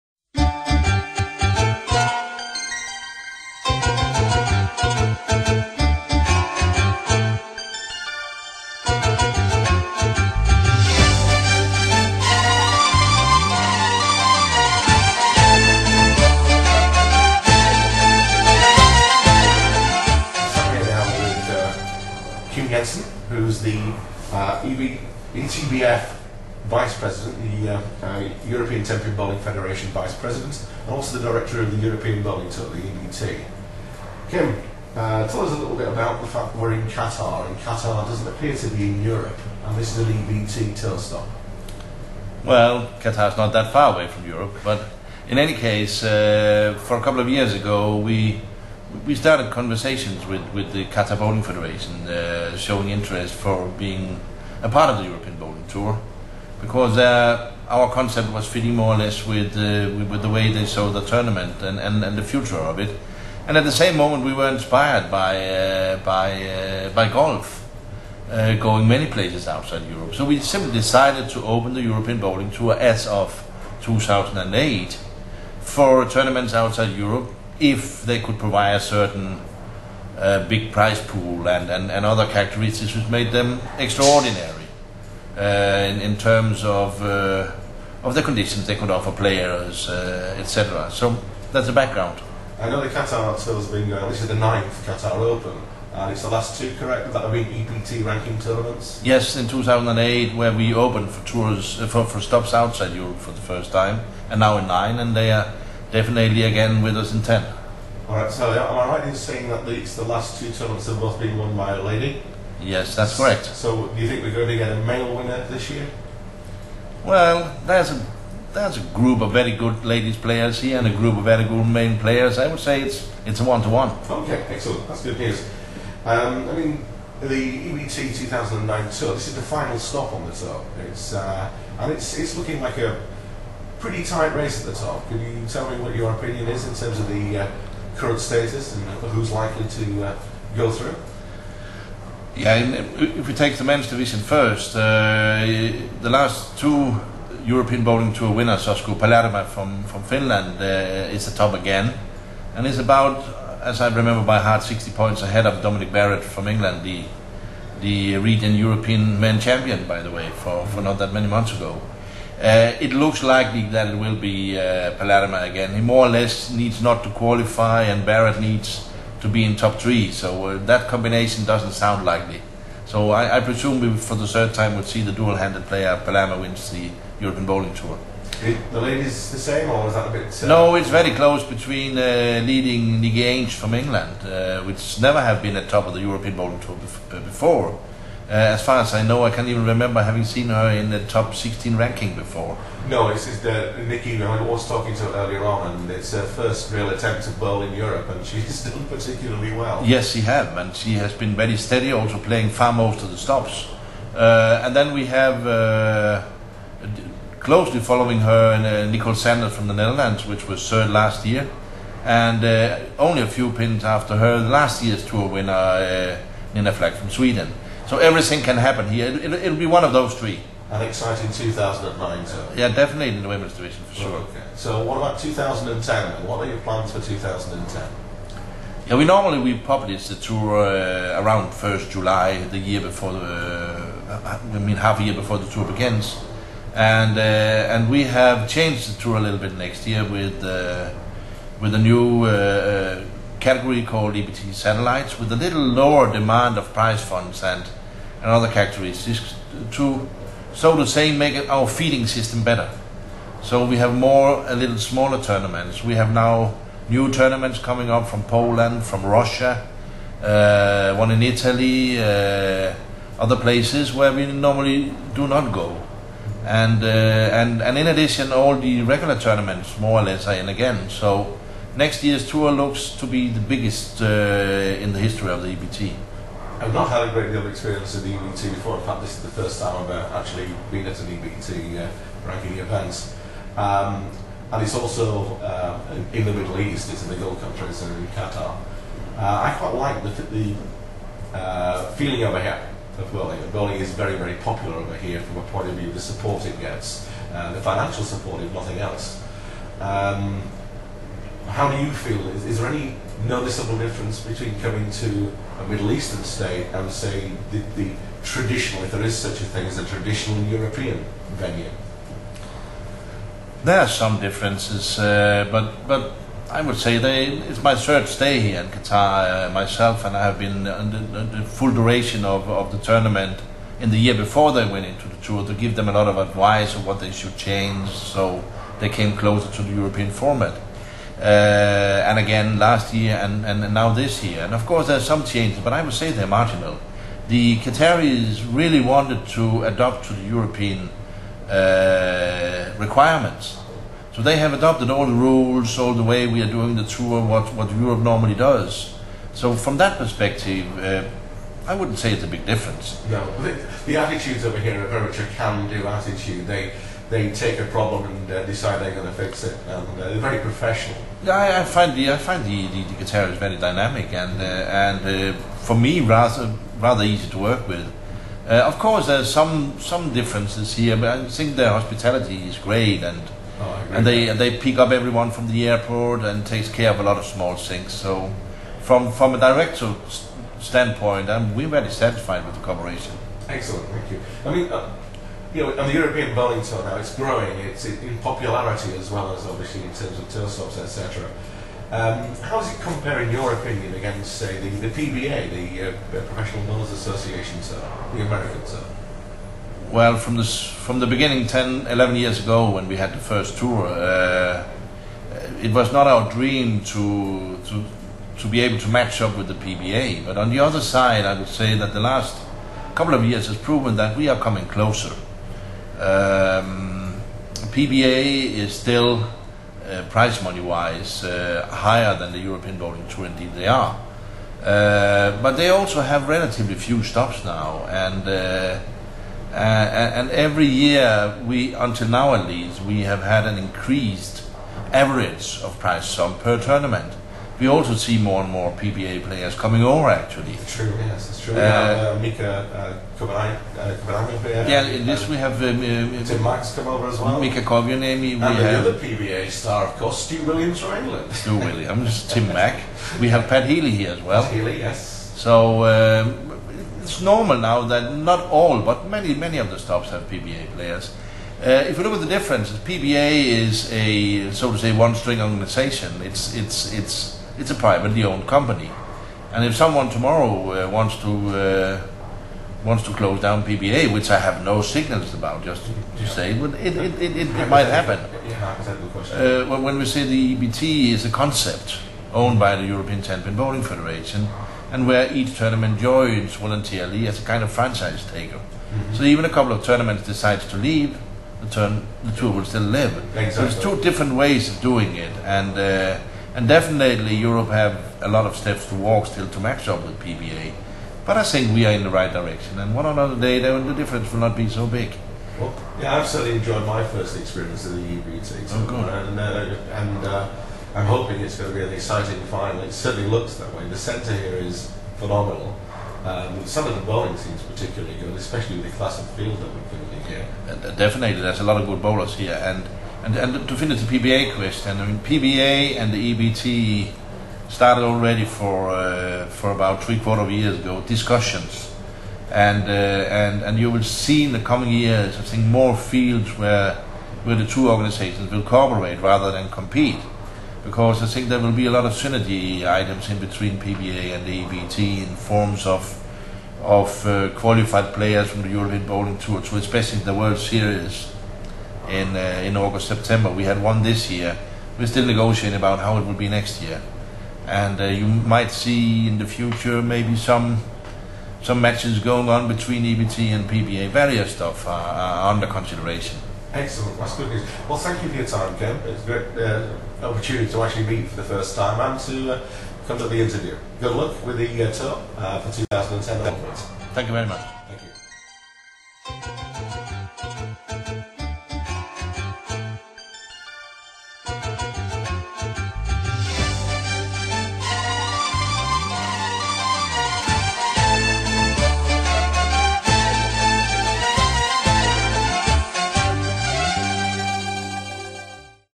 Qatar Bowling Open 2009 - Kim Jensen Talks Tenpin
Kim Thorsgard Jensen, EBTF Vice President and EBT Director, takes time out during the 15th and final tournament of the 2009 European Bowlers Tour (EBT), the 9th Qatar Bowling Open in December 2009, to to Talk Tenpin at the Qatar Bowling Center, Doha, Qatar.